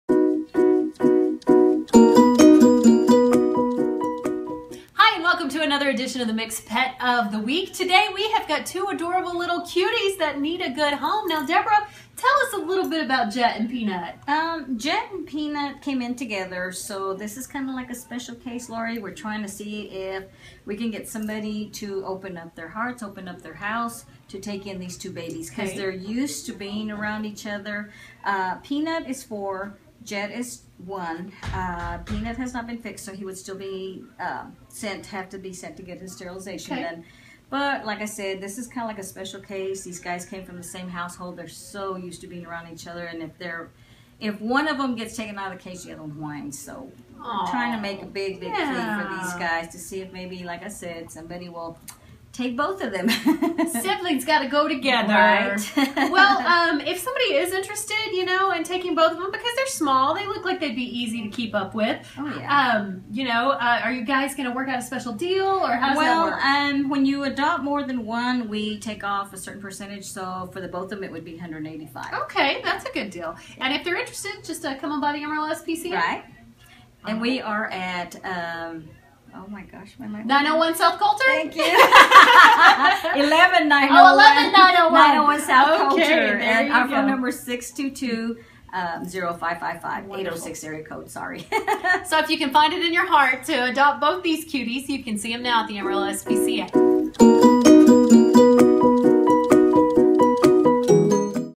Mix Pet of the Week -- Peanut and Jet
These two cute little dogs came in the shelter together and it would be best if they left that way. If you have a home for two dogs please contact the Amarillo SPCA!